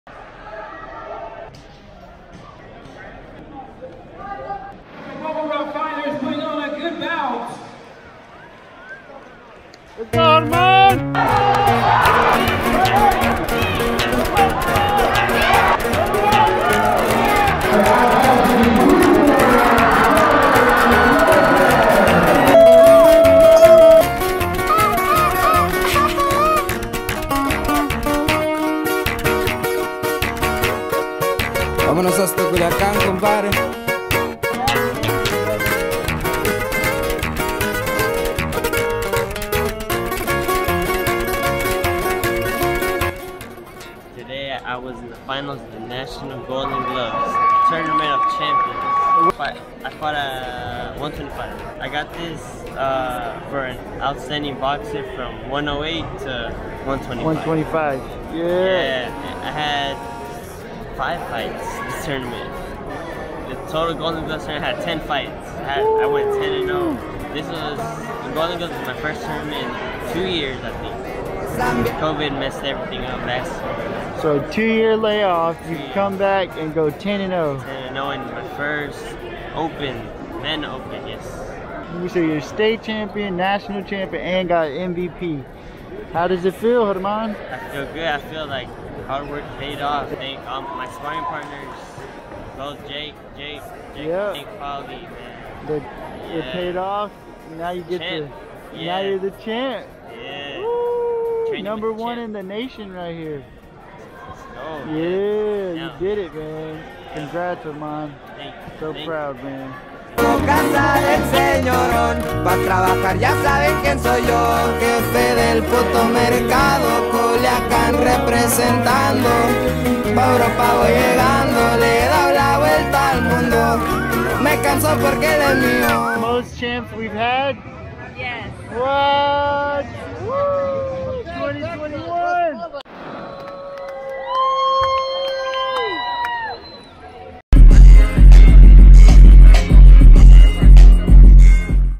PAULIE AYALA’S FIGHT TOWN EP 003 - German Lopez 2021 National Champion
German Lopez becomes the first Golden Gloves National Champion since 1989 representing Fort Worth, Texas!! And also winner of the Golden Gloves "Outstanding Boxer Award". Team Texas made history with 7 national champions as well as taking home the Team Trophy! German also became the first National champion out of Paulie Ayala's University of Hard Knocks Gym!!! Congrats German & and all team Texas National Champs!
108 lb Andres Perez
114 lb Figo Ramirez
125 lb Lizbeth Retiz
125 lb German Lopez
152 lb Giovanni Marquez
201 lb J'khory Gibson